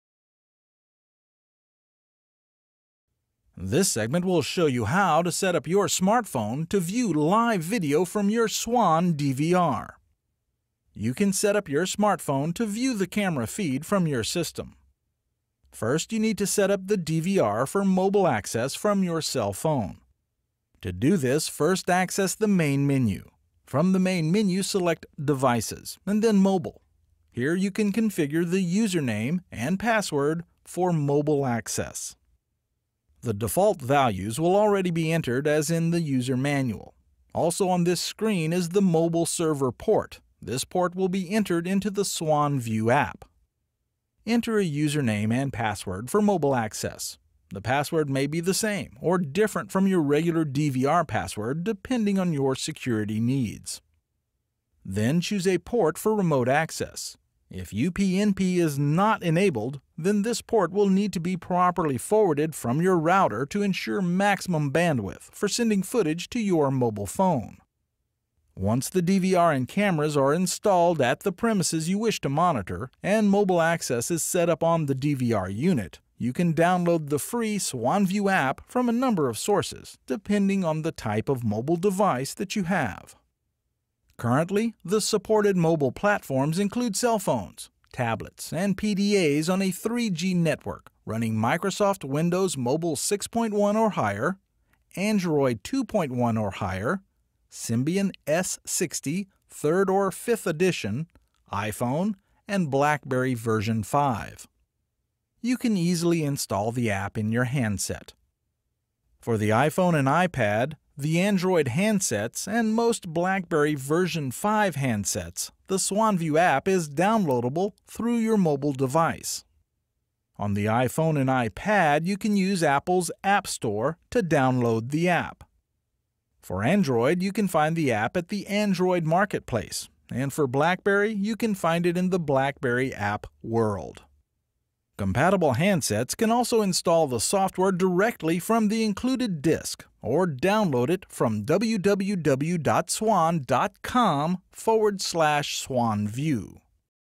Swann DVR CCTV - Camera Set-up For Smartphone Viewing
Swann Digital CCTV Security Camera System enables you to view your home or business from anywhere in the world via Internet access on your PC or your 3G-enabled Smartphone. The Swannview app can be viewed with iPhone, iPad, Blackberry, Android, Windows Mobile 6, Symbian OS and more.

Keep up-to-date!